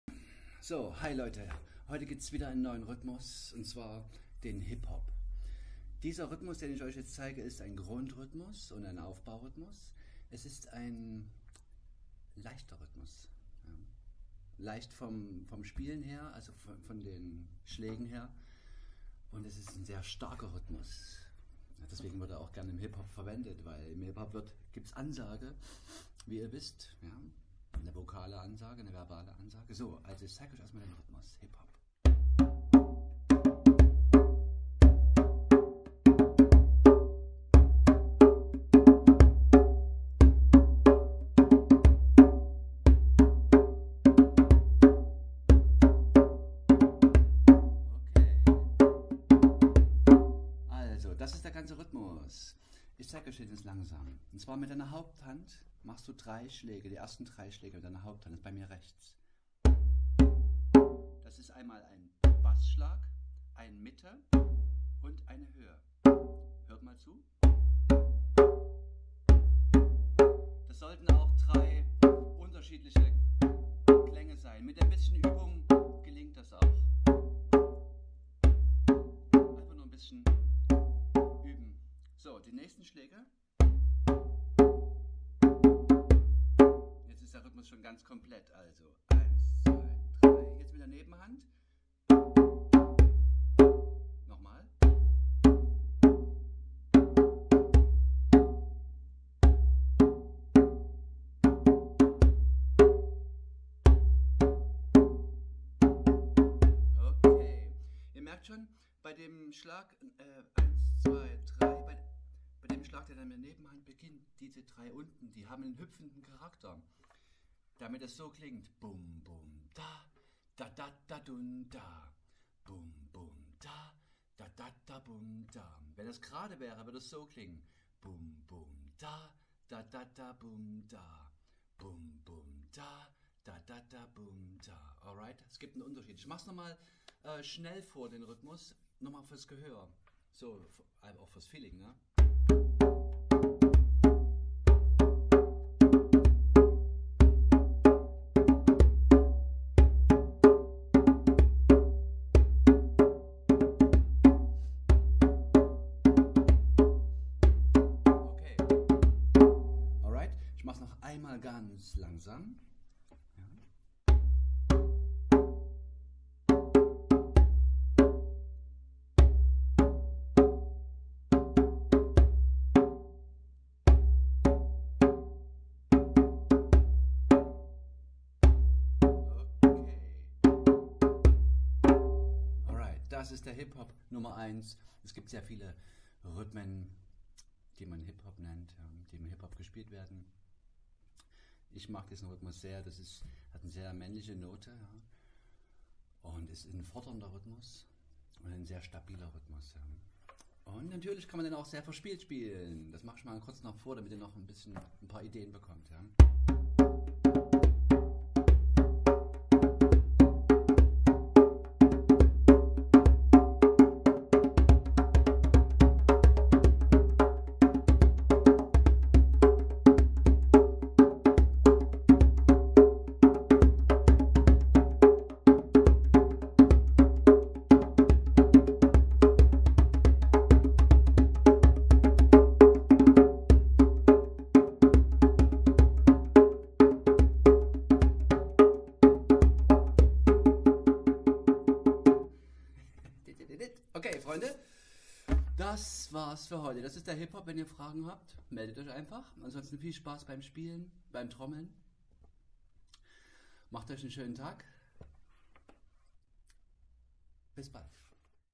Online Übung • Rhythmus "Hip Hop I"
Hi Leute, heute zeige ich euch den Rhythmus "Hip Hop I".

Es ist ein dynamischer Grund- und Aufbaurhythmus.

Yngo Gutmann